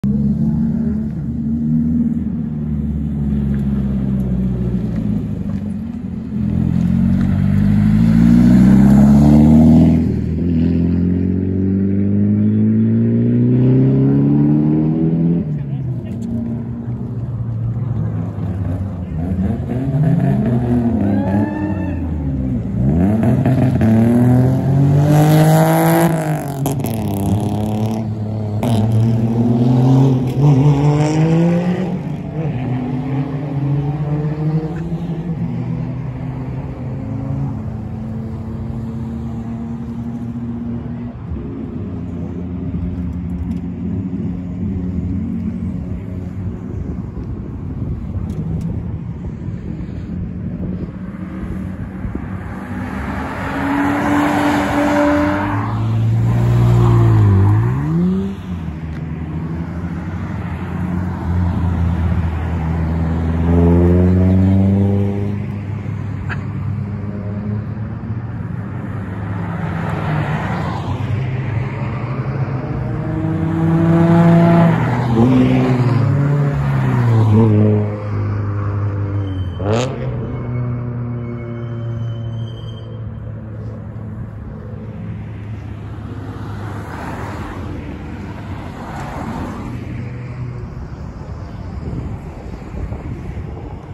某 公道ドリフト 警察官がパトロールカーで来てドリ車やギャラリーが散る。 基本的にギャラリーよりドリ車を先に行かせてあげた方が安全だし彼らも安心。
ギャラリー# ドリフト